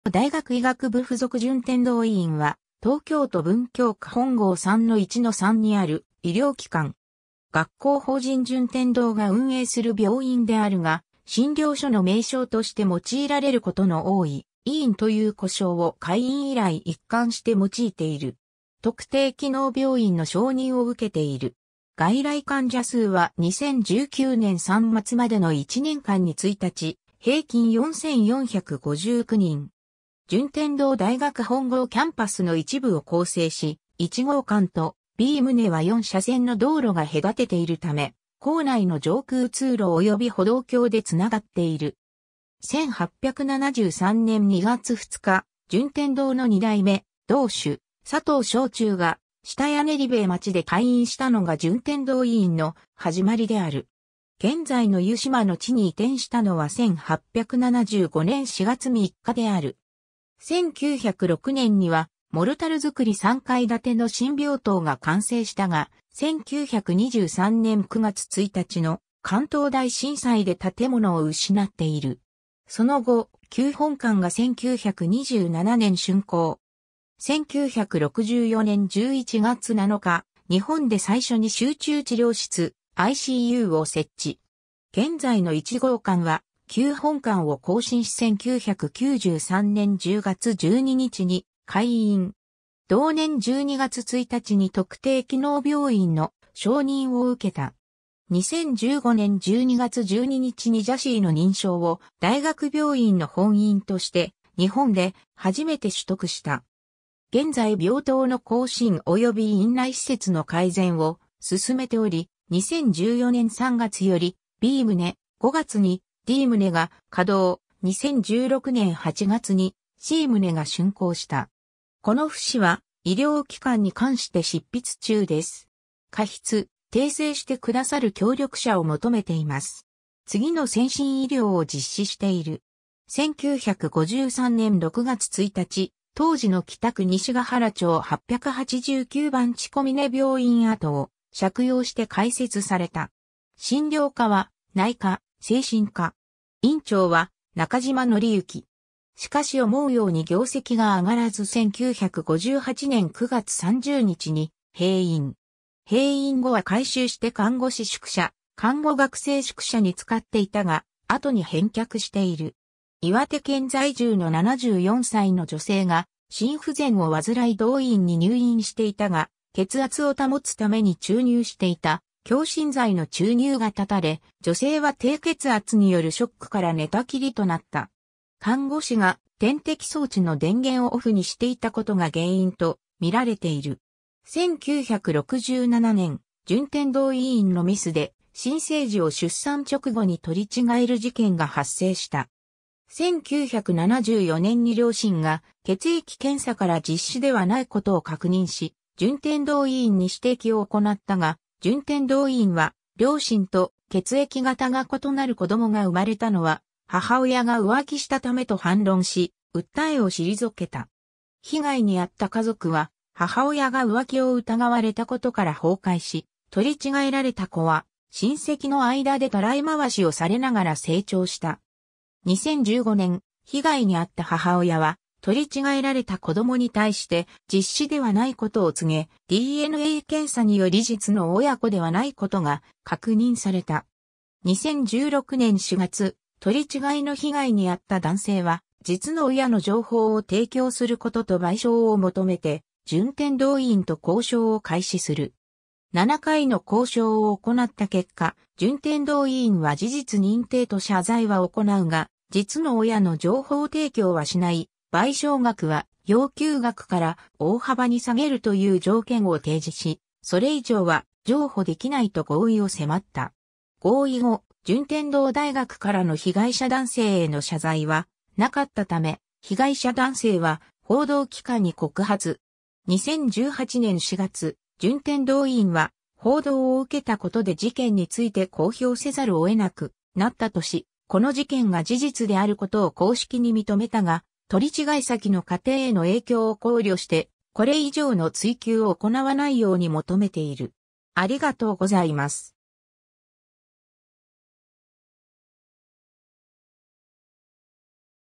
順天堂大学医学部附属順天堂医院
日本の医療機関_(私立学校法人)
日本の医療機関_(医育機関)
順天堂大学医学部附属順天堂医院（じゅんてんどうだいがくいがくぶふぞくじゅんてんどういいん、英語: Juntendo University Hospital）は、東京都文京区本郷3-1-3にある医療機関。
学校法人順天堂が運営する病院であるが、診療所の名称として用いられることの多い「医院（醫院）」という呼称を開院以来一貫して用いている。
特定機能病院の承認を受けている。
外来患者数は2019年3末迄の1年間に1日平均4459人。
 順天堂大学本郷キャンパスの一部を構成し、1号館とB棟は4車線の道路（文京区道836号）が隔てているため、構内の上空通路及び歩道橋で繋がっている。
 1873年（明治6年）2月2日、順天堂の2代目堂主佐藤尚中が下谷練塀町で開院したのが順天堂医院の始まりである。
現在の湯島の地に移転したのは1875年（明治8年）4月3日である。
1906年（明治39年）にはモルタル造3階建ての新病棟が完成したが、1923年（大正12年）9月1日の関東大震災で建物を失っている。
その後、旧本館が1927年（昭和2年）竣工。
1964年（昭和39年）11月7日、日本で最初に集中治療室:ICUを設置。
現在の1号館は旧本館を更新し1993年10月12日に開院。
同年12月1日に特定機能病院の承認を受けた。
2015年12月12日にJCI(Joint Commission International)の認証を大学病院の本院として日本で初めて取得した。
 現在病棟の更新及び院内施設の改善を進めており、2014年3月よりB棟(2016年竣工)、5月にD棟が稼動、2016年8月にC棟が竣工した。
 この節は医療機関に関して執筆中です。
加筆、訂正して下さる協力者を求めています（PJ医療機関）。
 次の先進医療を実施している。
 1953年6月1日、当時の北区西ヶ原町889番地小峰病院（精神科病院）跡を借用して開設された。
診療科は内科・精神科。
院長は中島紀行（内科学）。
しかし思うように業績が上がらず1958年9月30日に閉院。
閉院後は改修して看護師宿舎・看護学生宿舎に使っていたが、後に返却している。
 岩手県在住の74歳の女性が、心不全を患い同医院に入院していたが、血圧を保つために注入していた強心剤の注入が断たれ、女性は低血圧によるショックから寝たきりとなった。
看護師が点滴装置の電源をオフにしていたことが原因と見られている。
 1967年、順天堂医院のミスで新生児を出産直後に取り違える事件が発生した。
1974年に両親が血液検査から実子ではないことを確認し、順天堂医院に指摘を行ったが、順天堂医院は両親と血液型が異なる子供が生まれたのは母親が浮気したためと反論し、訴えを退けた。
被害にあった家族は母親が浮気を疑われたことから崩壊し、取り違えられた子は親戚の間でたらい回しをされながら成長した。
 2015年、被害にあった母親は、取り違えられた子供に対して実子ではないことを告げ、DNA検査により実の親子ではないことが確認された。
 2016年4月、取り違えの被害にあった男性は、実の親の情報を提供することと賠償を求めて、順天堂医院と交渉を開始する。
7回の交渉を行った結果、順天堂医院は事実認定と謝罪は行うが、実の親の情報提供はしない、賠償額は要求額から大幅に下げるという条件を提示し、それ以上は譲歩できないと合意を迫った。
 合意後、順天堂大学からの被害者男性への謝罪はなかったため、被害者男性は報道機関に告発。
2018年4月、順天堂医院は報道を受けたことで事件について公表せざるを得なくなったとし、この事件が事実であることを公式に認めたが、取り違え先の家庭への影響を考慮して、これ以上の追及を行わないように求めている。
 過去には社会的に影響力のある患者も受け入れる特別病室があり、椎名悦三郎などの政治家らが入院したことがある。
1976年にはロッキード事件の渦中にあった実業家の小佐野賢治が入院。
病室には偽名の名札が掲げられて面会謝絶の措置が採られた。
当時の差額ベッド代は日額4万円を超える額となっていた。